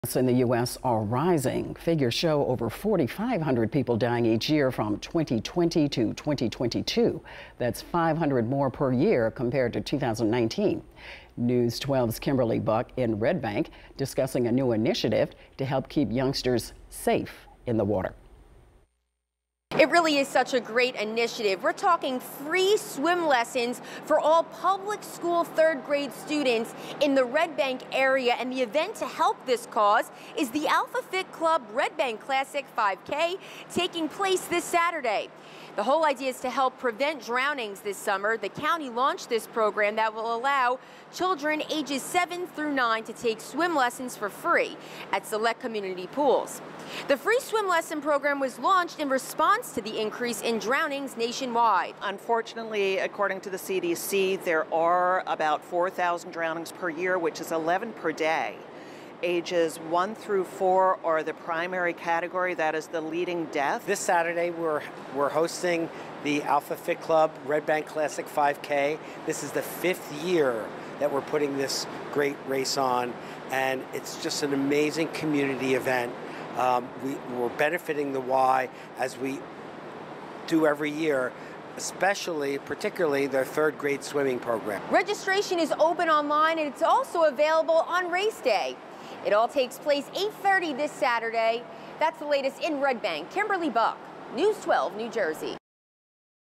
Drowning Prevention - News 12 NJ
Kimberly Buk reports from Red Bank.